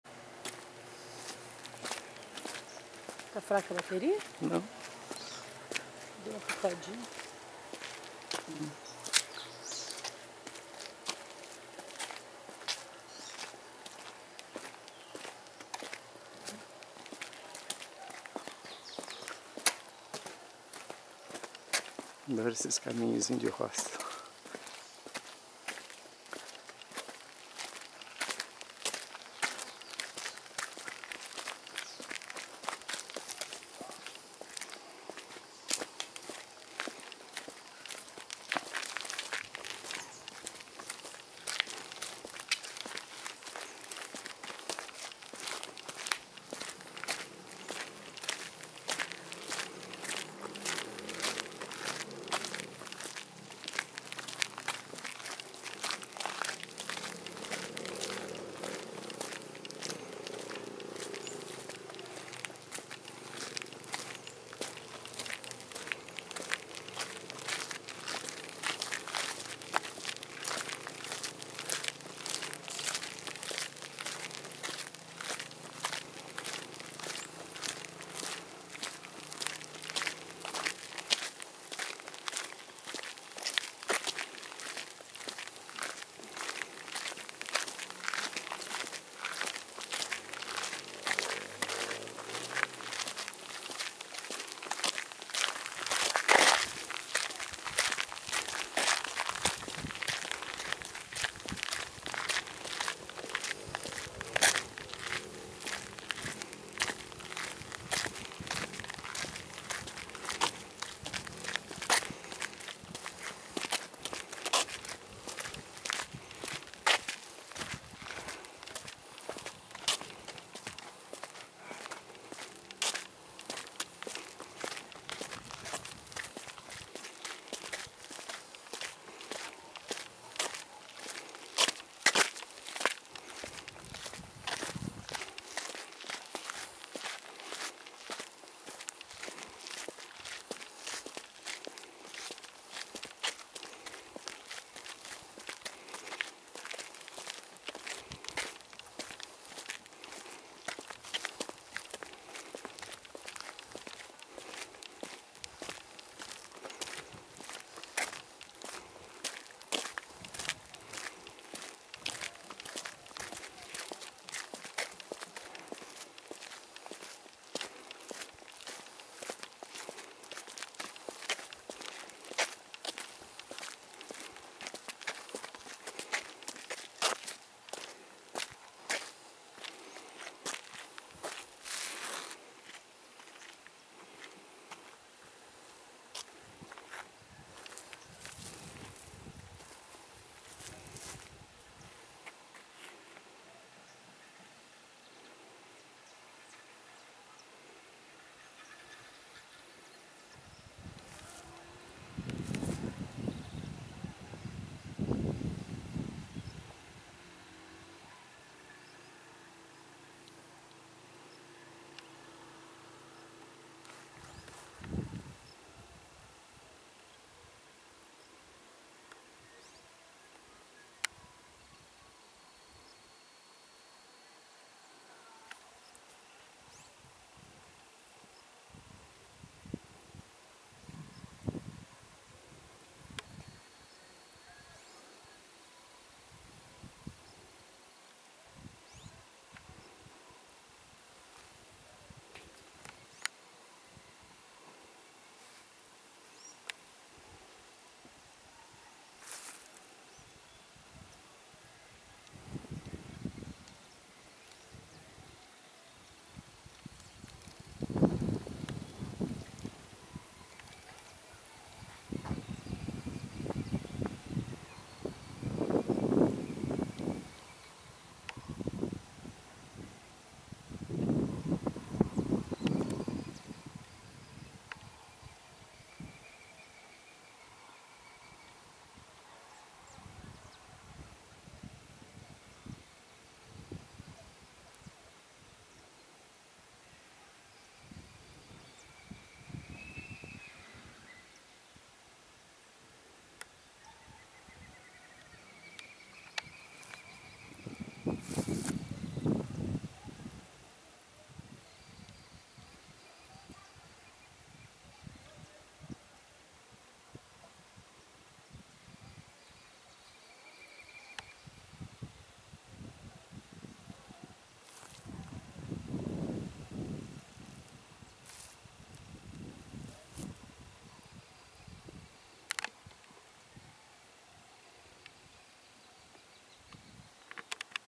São Pedro do Itabapoana, passeio matinal 1/2
Passeamos pela estrada vicinal que passa pelo campo santo, uma linda manhã ensolarada do dia 21 de abril de 2017, uma sexta-feira.